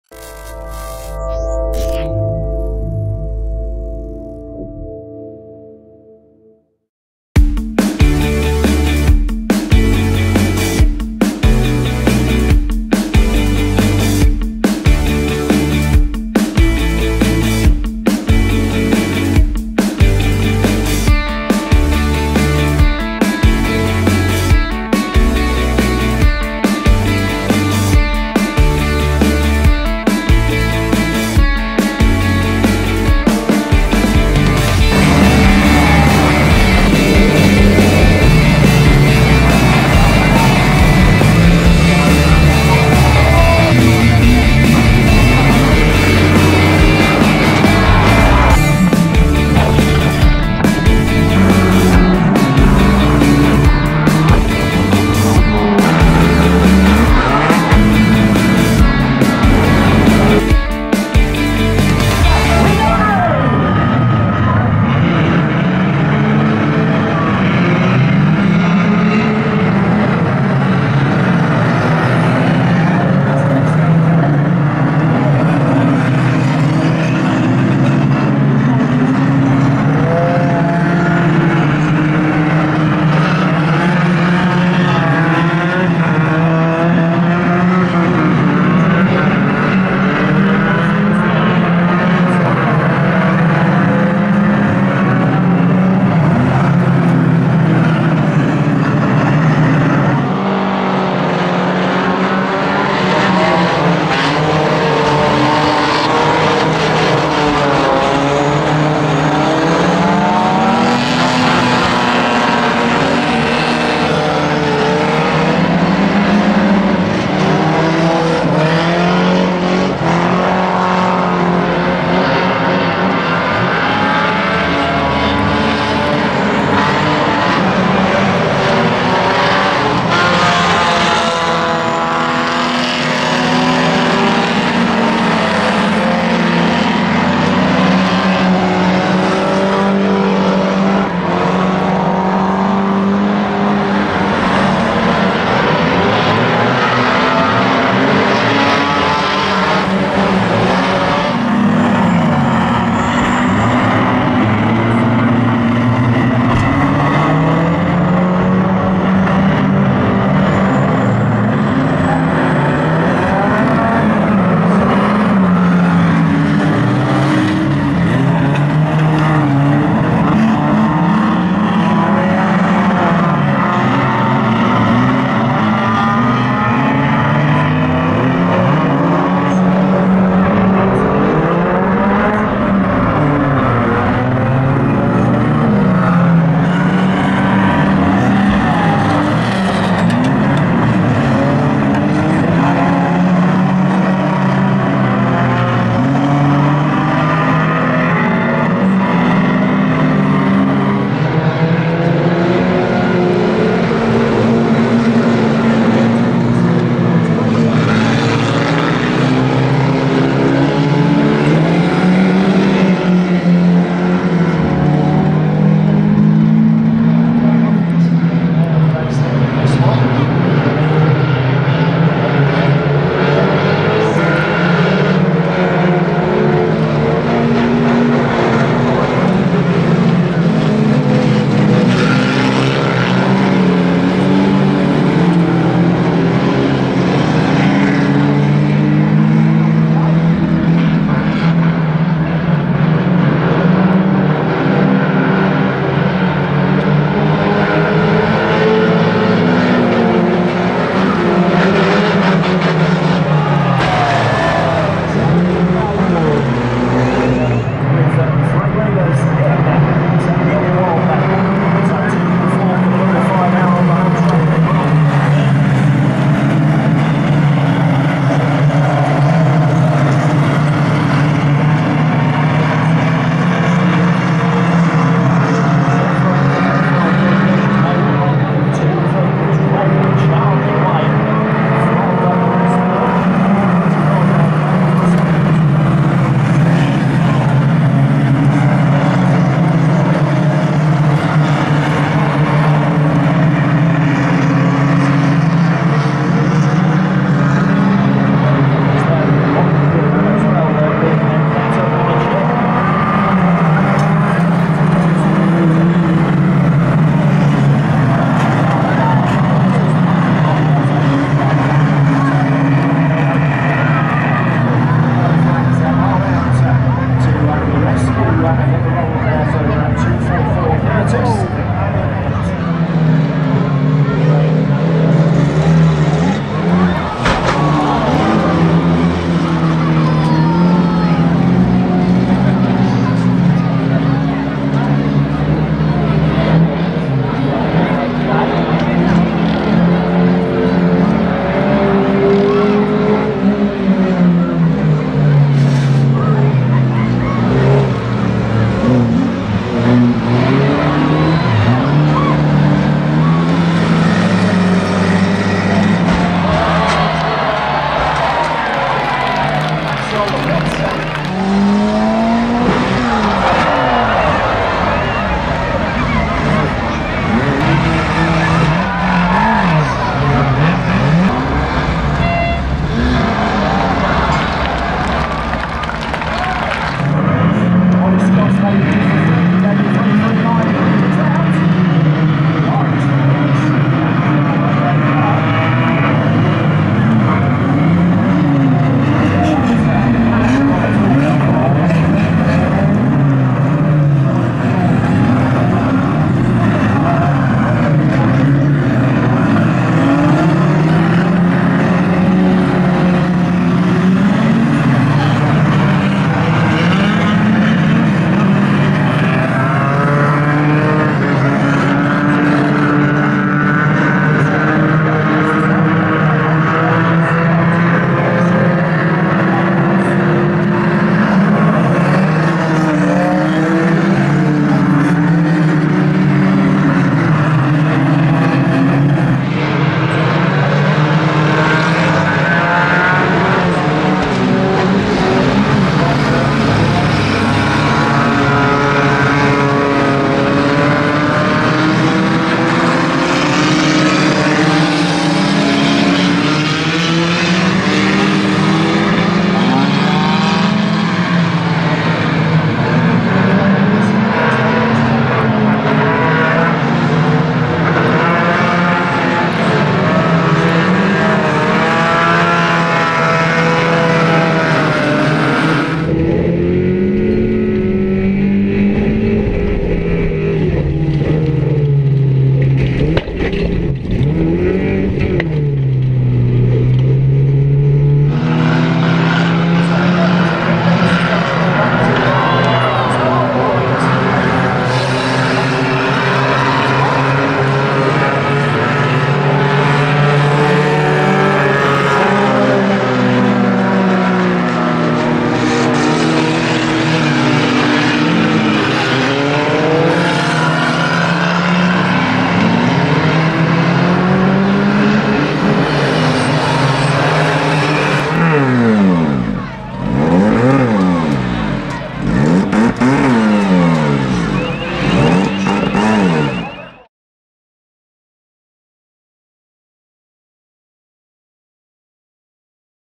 Kings Lynn Easter Monday 2022 Wacky Races 4X4 Bangers Siamese Bangers Reliant Robins Impact Videos
A selection of clips from the Easter Monday Wacky Races event at Kings Lynn. Featuring hard hitting 4X4 SUV Bangers, Spectacular Siamese Bangers, lively Micro Banger action and rollover fun with Reliant Robin 3 wheelers. The full DVD production features every race with a 2 camera edit, live race commentary, pit and wreck shots, in-car video and action replay.